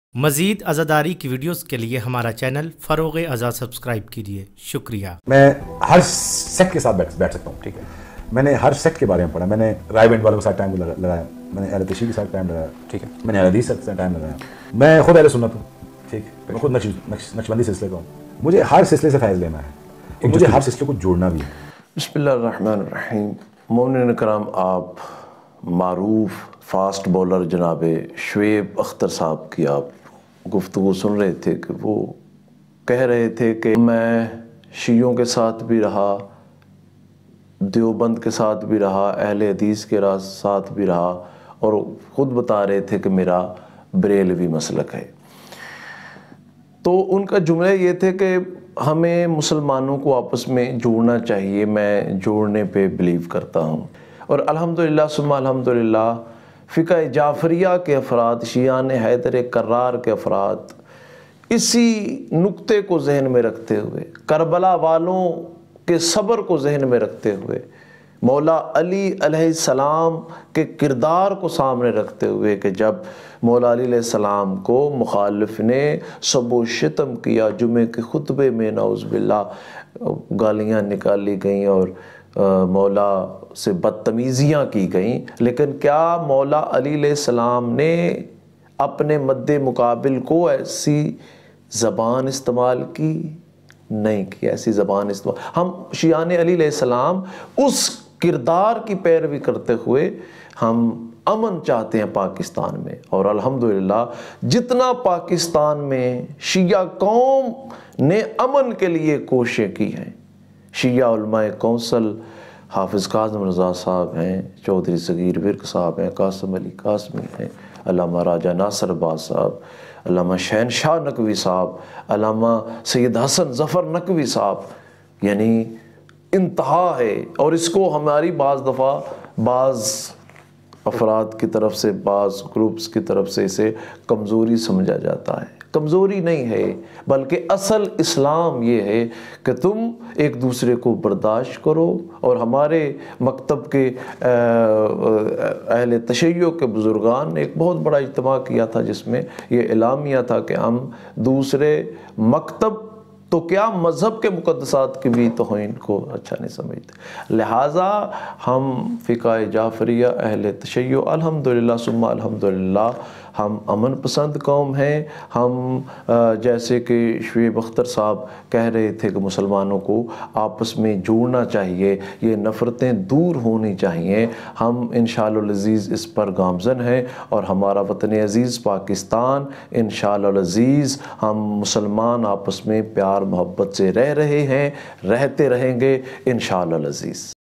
Shoaib Akhtar ny Shia Moqaf Ki Taeed Kardi Allama Safdar Ali
Shoaib Akhtar ny Itehad e Ummat ke Hawaly se Shia Moqaf ki taieed kar di
Speaker : Allama Safdar Ali